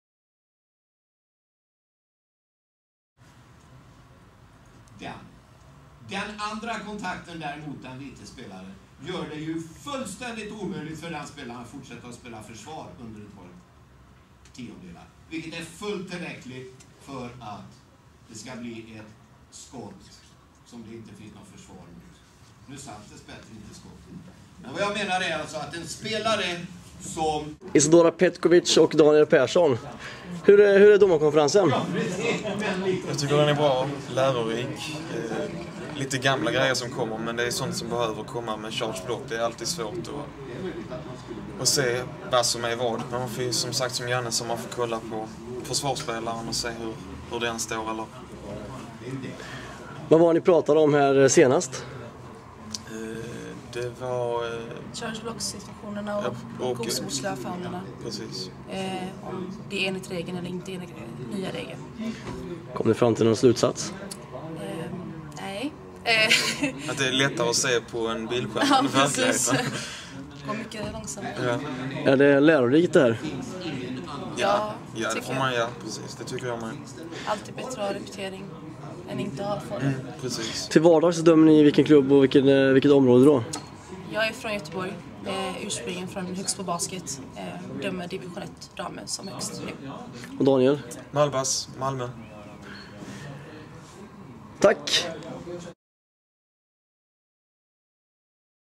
Domarkonferensen
Samtidigt som Ledarforum pågår håller även Domarna en konferens.Isadora Petcovic och Daniel Person var två av deltagarna.